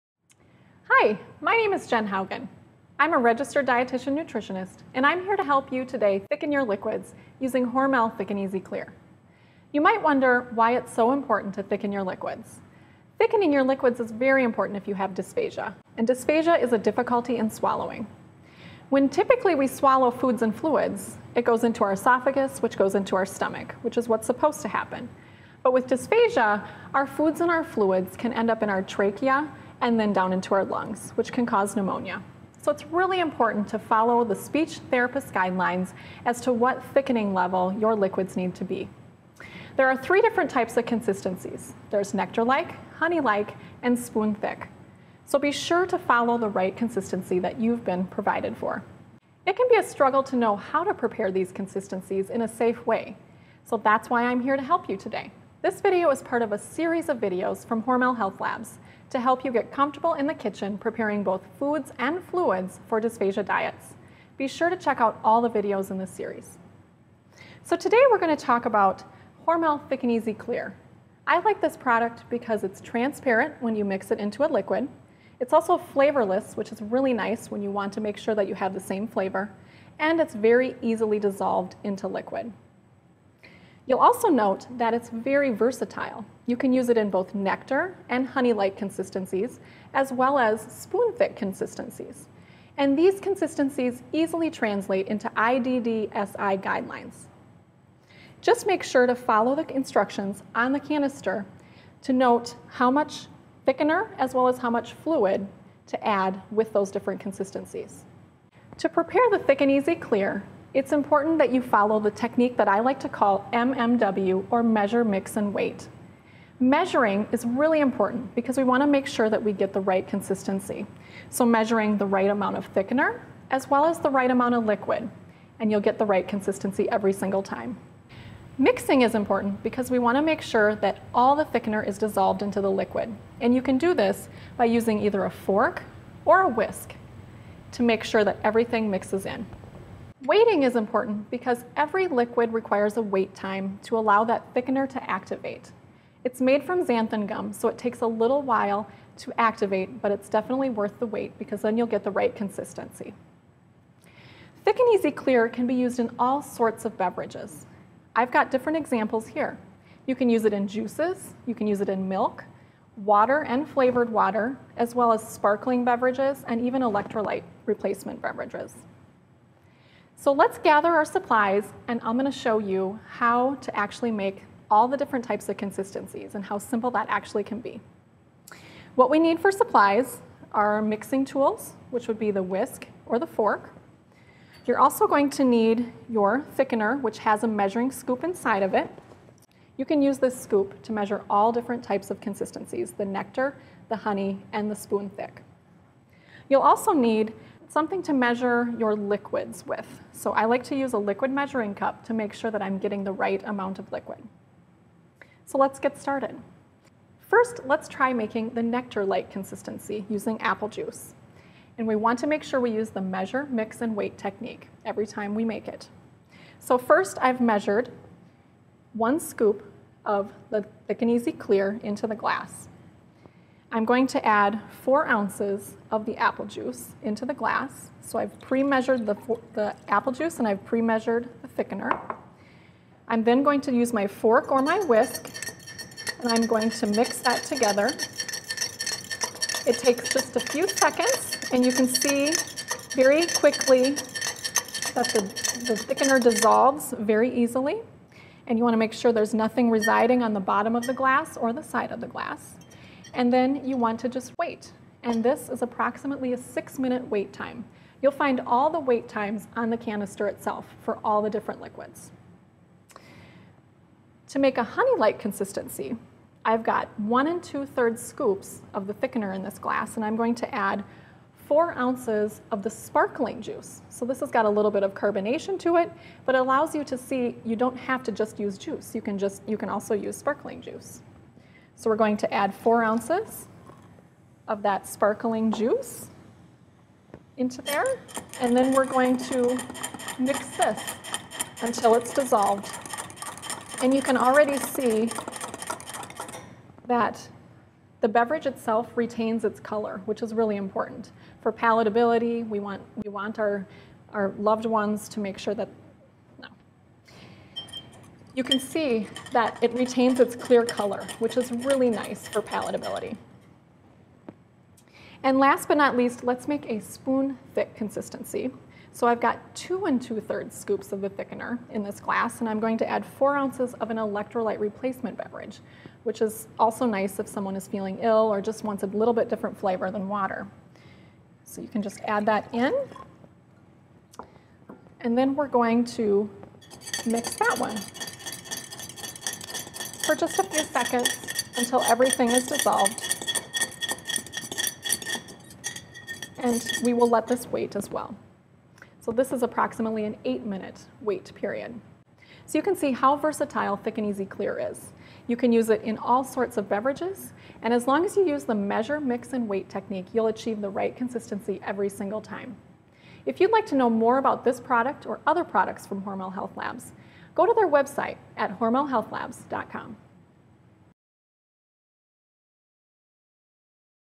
How to Thicken Your Liquids Using HORMEL® THICK & EASY® Clear
Jen Haugen, RDN, explains how to thicken your liquids to a level 4 consistency using HORMEL® THICK & EASY® clear.